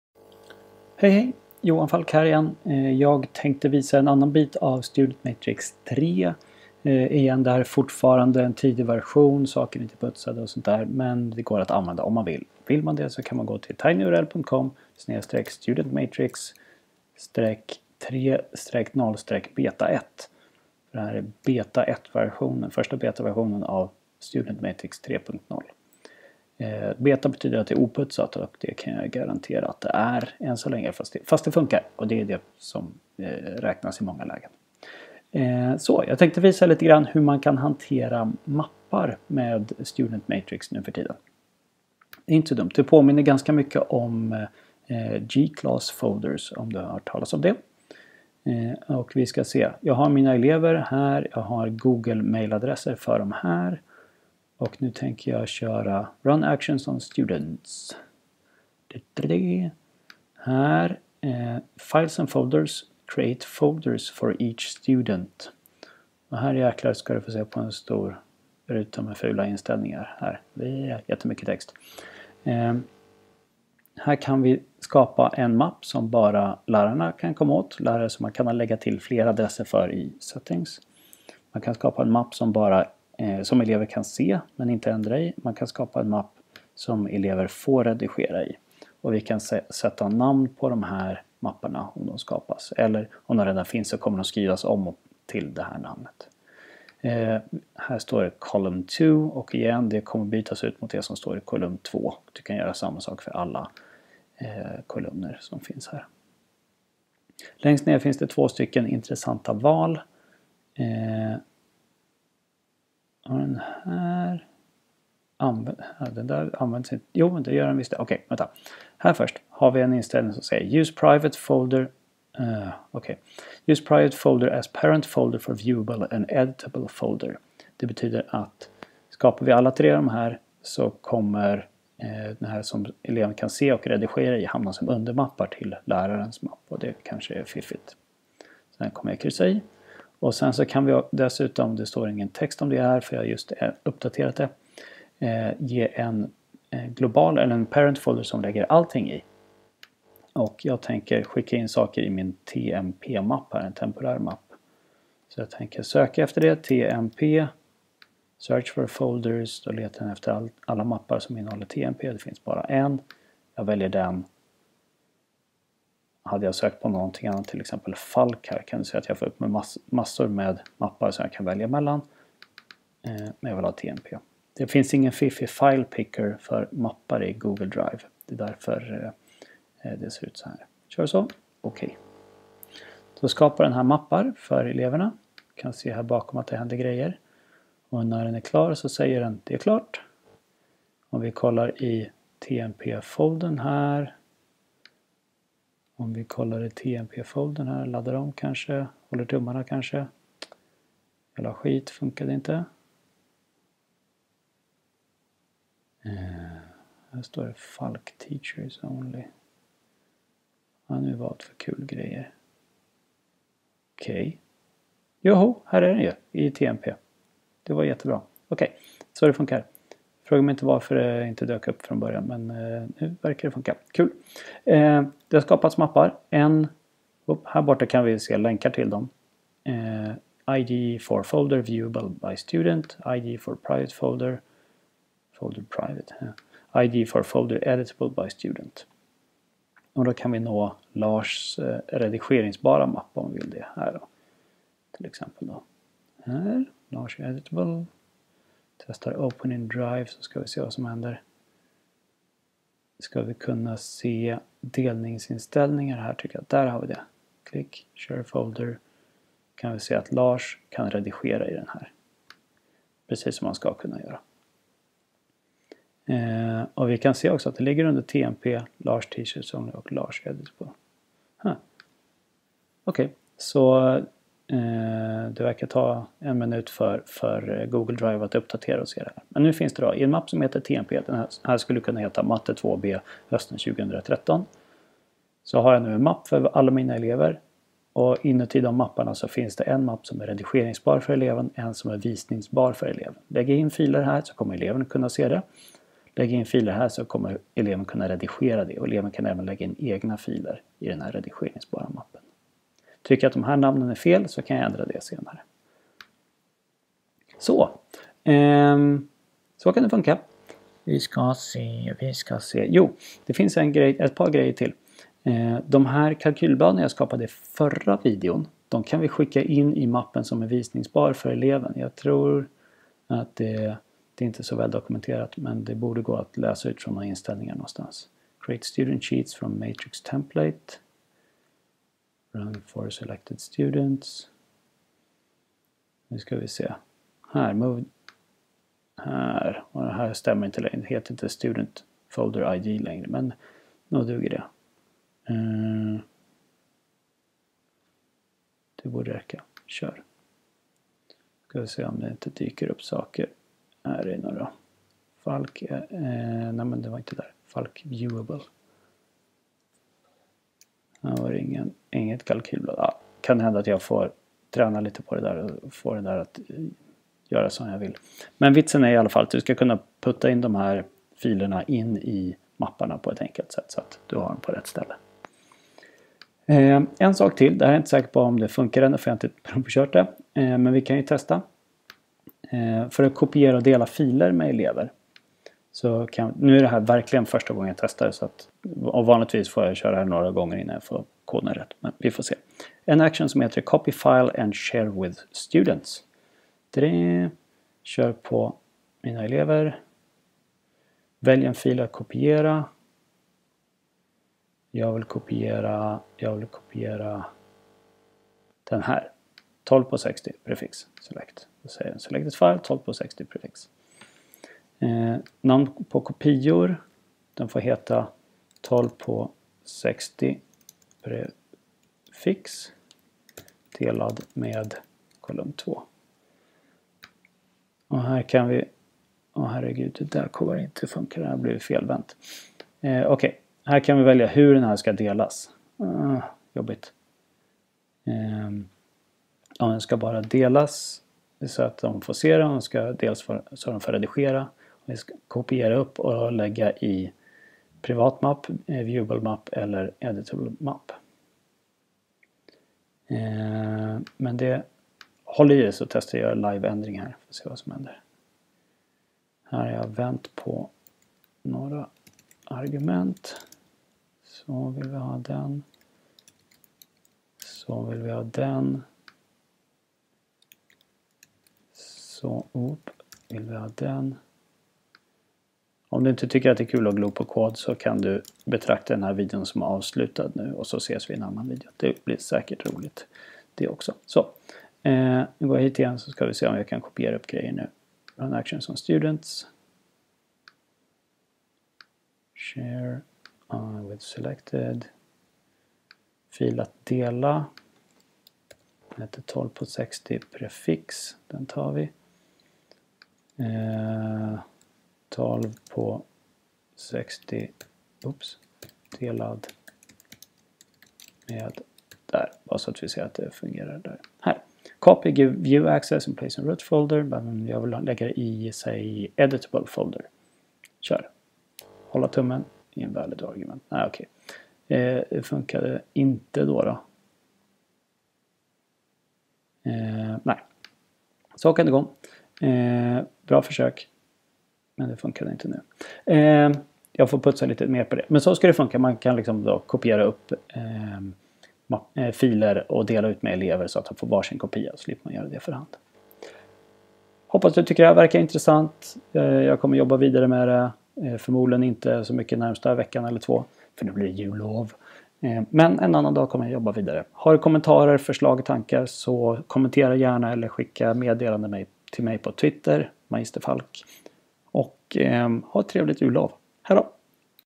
StudentMatrix 3.0-beta1: skapa mappar
Den här videon visar en första betaversion av StudentMatrix 3. I videon syns hur man skapar mappar för sina elever, för att enklare dela filer med dem.